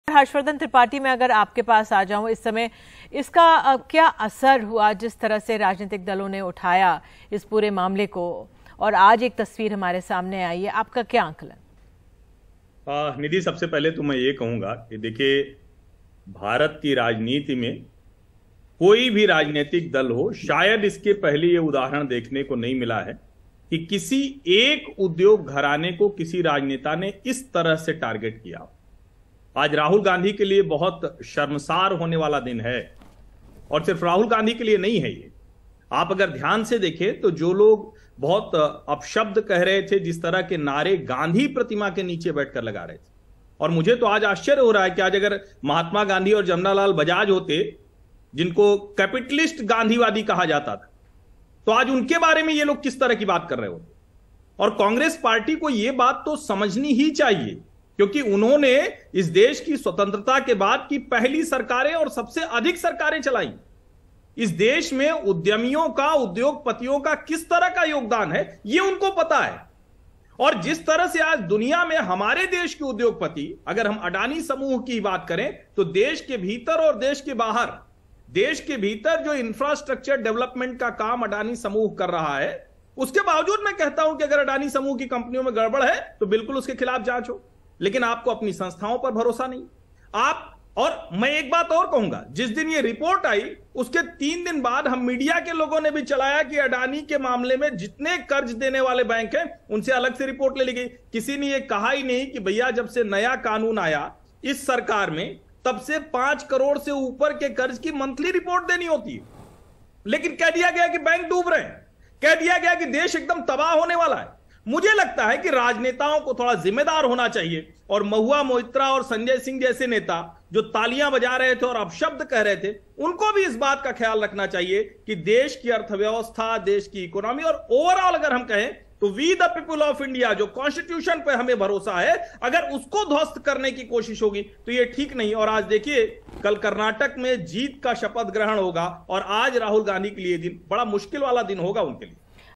"Opposition नेताओं को जिम्मेदार होना चाहिए": वरिष्ठ पत्रकार हर्षवर्धन त्रिपाठी | Des Ki Baat
हिंडनबर्ग केस (Hindenburg Case) में सुप्रीम कोर्ट (Supreme Court) की एक्सपर्ट कमेटी ने अदाणी ग्रुप को क्लीन चिट दे दी है. रिपोर्ट में कहा गया है कि अदाणी ग्रुप (Adani Group) ने किसी भी कानून का उल्लंघन नहीं किया गया है. शेयर प्राइस में अदाणी ग्रुप ने कोई हेरफेर नहीं की. SEBI को कीमतों में बदलाव की पूरी जानकारी थी. वरिष्ठ पत्रकार हर्षवर्धन त्रिपाठी ने इस पूरे मामले को लेकर कांग्रेस और विपक्षी पार्टियों के नेता पर हमला बोला है. उन्होंने कहा कि एक उद्योग घराने को टारगेट किया गया. विपक्षी नेताओं को जिम्मेदार होना चाहिए.